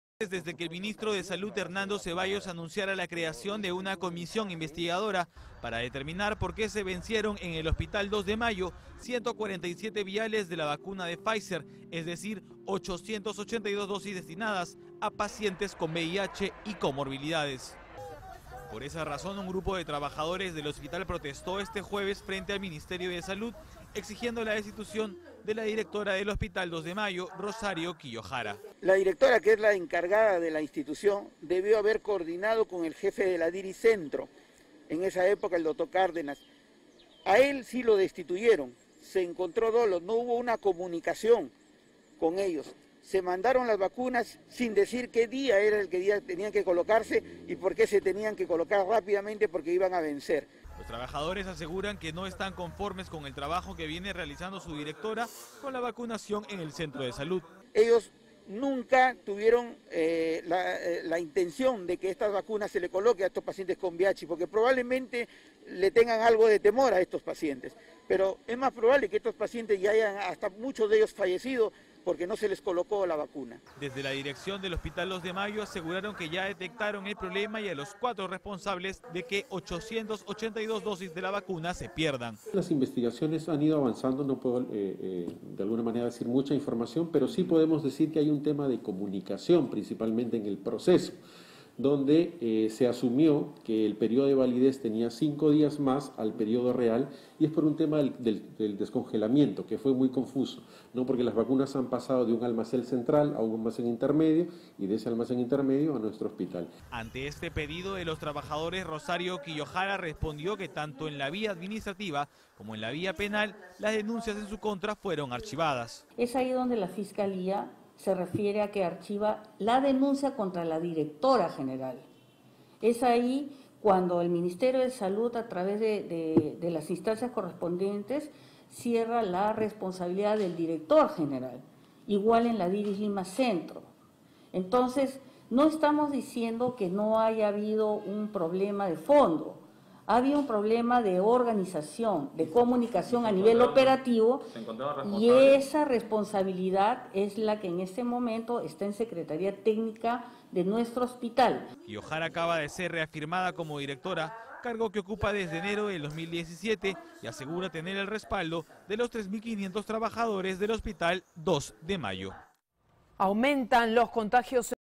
Trabajadores de Hospital Dos de Mayo exigen renuncia de directora
Han pasado 5 meses desde que el ministro de Salud, Hernando Cevallos, anunciara la creación de una comisión investigadora para determinar porqué se vencieron 147 viales de la vacuna de Pfizer, es decir, 882 dosis destinadas a pacientes con VIH y comorbilidades.

Por esa razón, un grupo de trabajadores del hospital protestó este jueves frente al Ministerio de Salud exigiendo la destitución de la directora del Hospital Dos de Mayo, Rosario Kiyohara.

Los trabajadores aseguran que no están conformes con el trabajo que viene realizando su directora con la vacunación en el centro de salud.

Ante este pedido de los trabajadores, Rosario Kiyohara respondió que tanto en la vía administrativa como en la vía penal las denuncias en su contra fueron archivadas.

Kiyohara acaba de ser reafirmada como directora, cargo que ocupa desde enero del 2017 y asegura tener el respaldo de los 3500 trabajadores del Hospital Dos de Mayo.

Desde la dirección del Hospital Dos de Mayo aseguraron que ya detectaron el problema y a los 4 responsables de que 882 dosis de la vacuna se pierdan.